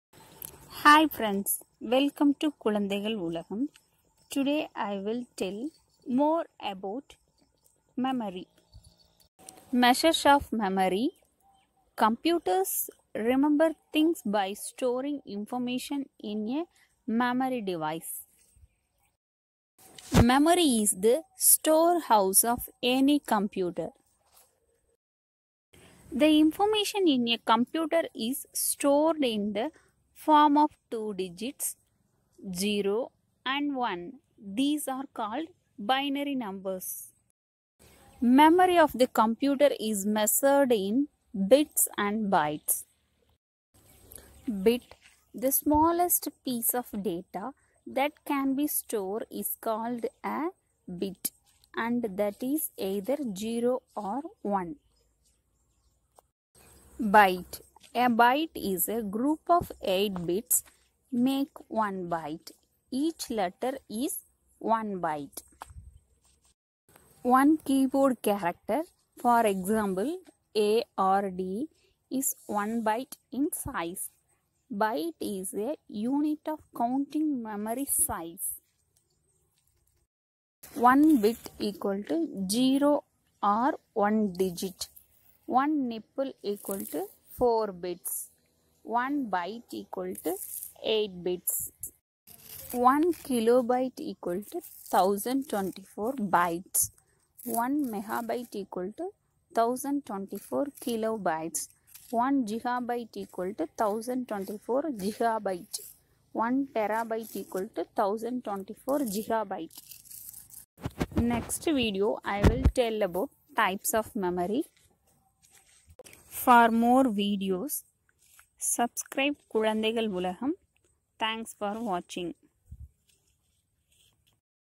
More about memory | computer memory | measures of memory | bit| grade 4 | Kulanthaigal Ulagam | KU
This video tell about
Computer memory is a generic term for all of the different types of data storage technology that a computer may use, including RAM, ROM, and flash memory.
bit, nibble, byte, kb, tb, gb,mb